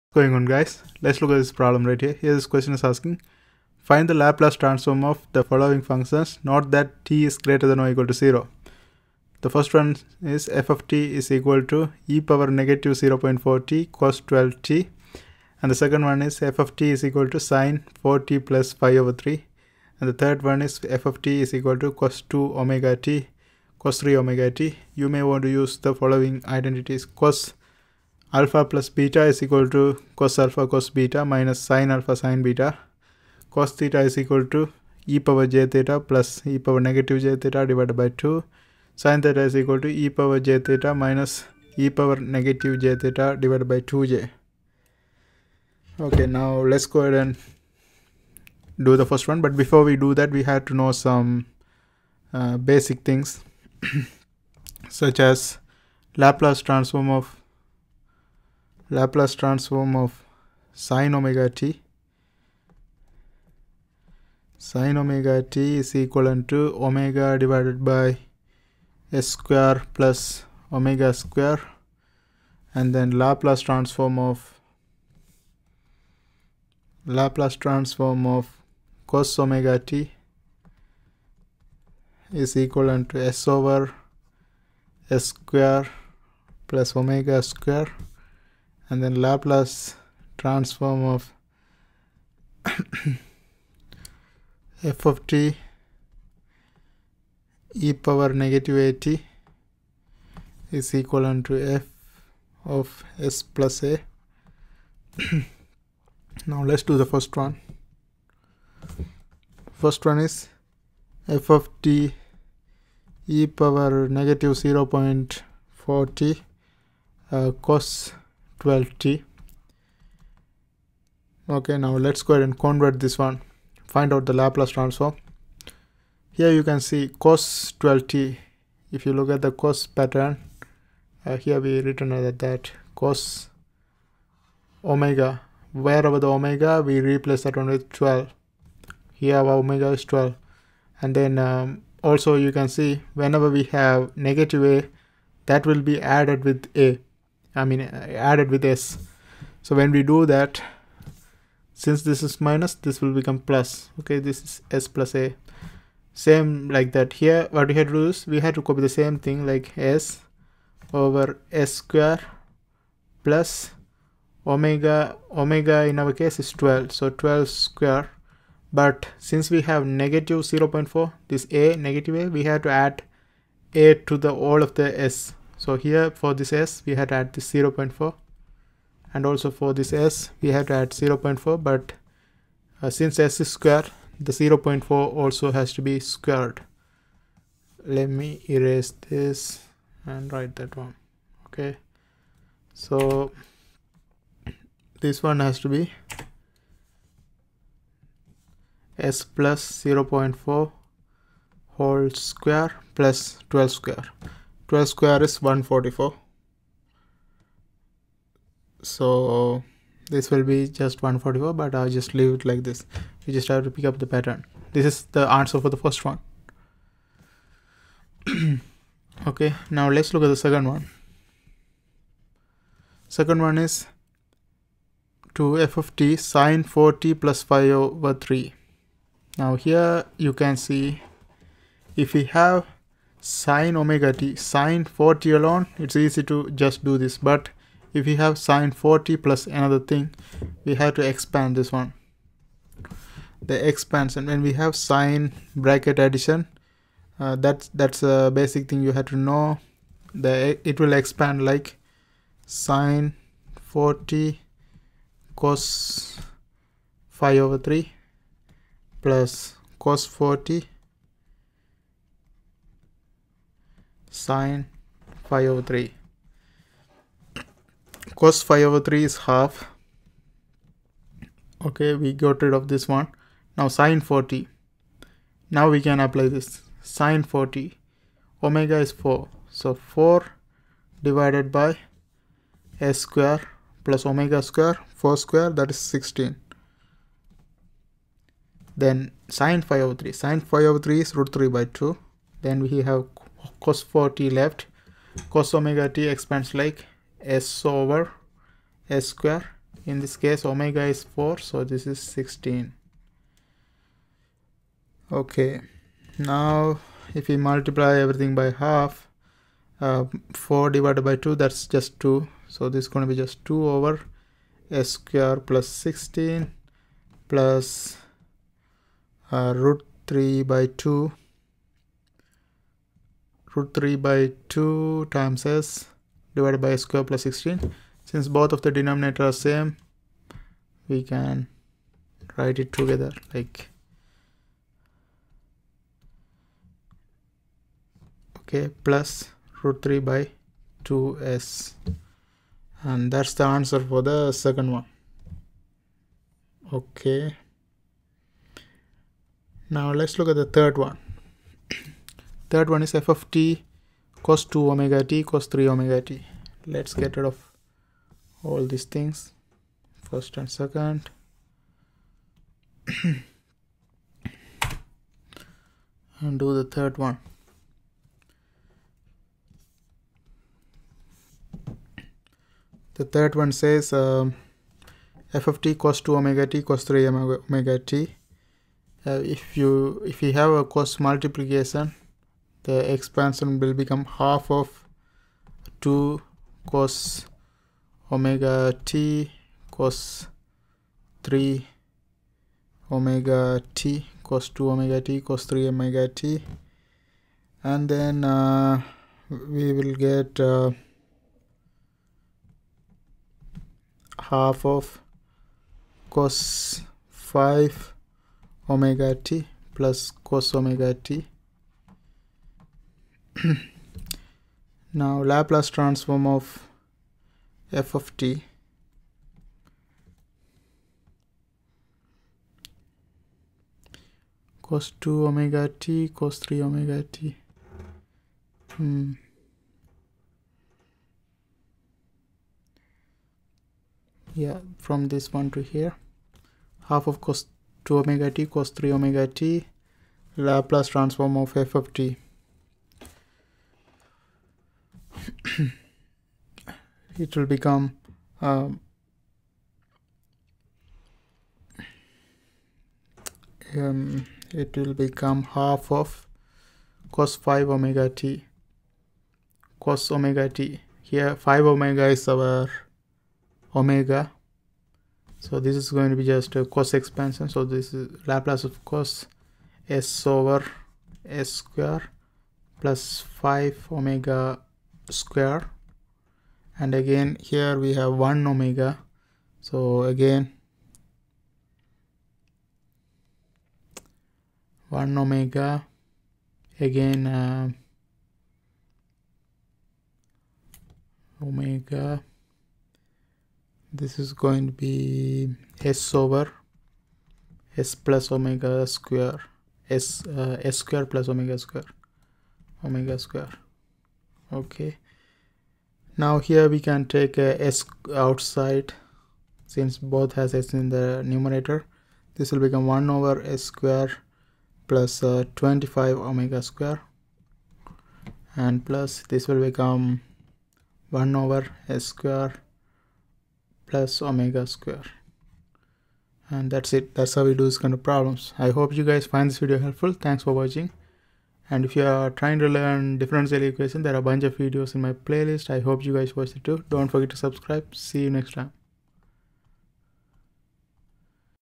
Find the Laplace transforms of the following functions
Find the Laplace transforms of the following functions: note that t ≥ 0,
1). f(t) = e
−0.4t
cos12t.
2). f(t) = sin(4t +
π
3
3). f(t) = cos2ωtcos3ωt.
Hint: You may want to use the following identities:
cos(α + β) = cosαcosβ − sinαsinβ,
cos θ =
e
jθ + e
−jθ
2
, sin θ =
e
jθ − e
−jθ
2j